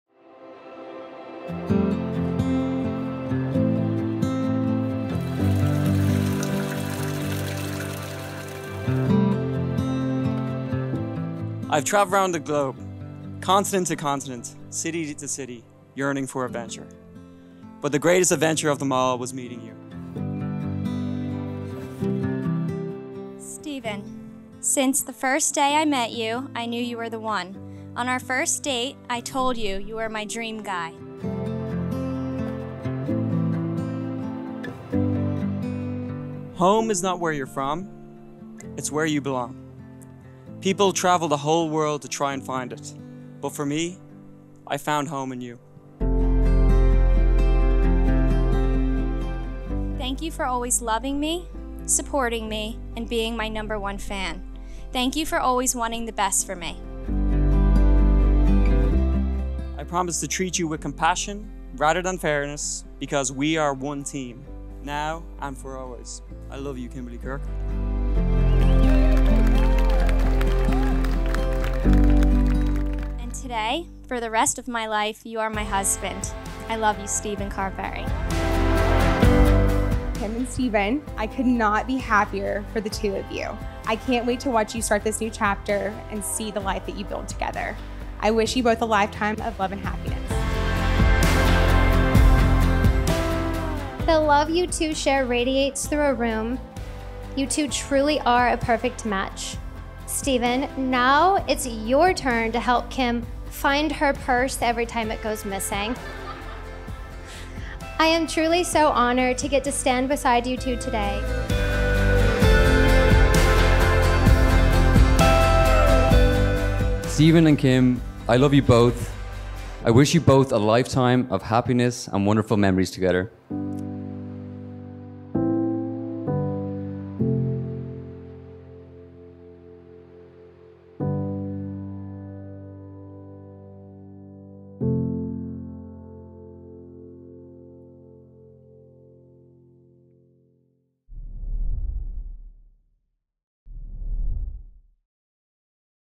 Kimberley and Stephen's Captivating Wedding Film from Doylestown, Pennsylvania
Kimberley and Stephen's wedding at the historic Aldie Mansion was like a fairytale. This venue offered the perfect backdrop to their elegant and thoughtful wedding. Stephen's vows to Kimberley included a beautiful line about how how the term "home" was found in her. This outdoor wedding was full of elegance, charm and love between this beautiful couple.

Wedding Venue: Aldie Mansion, Doylestown, Pennsylvania
Wedding Photography: Yana Shellman Photography
Wedding DJ: Unbreakable Entertainment